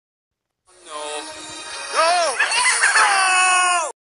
My reaction when Arsen54800 got closed and terminated
No! No! Nooooooooooo! Why Arsen54800 haters (CYSDN, Clancy the Awesome Dude 2!, Joseph Goes UTTP, etc.) closed his account?

Note: I like Shrek. CYSDN = Christmas Yes Sunny Day No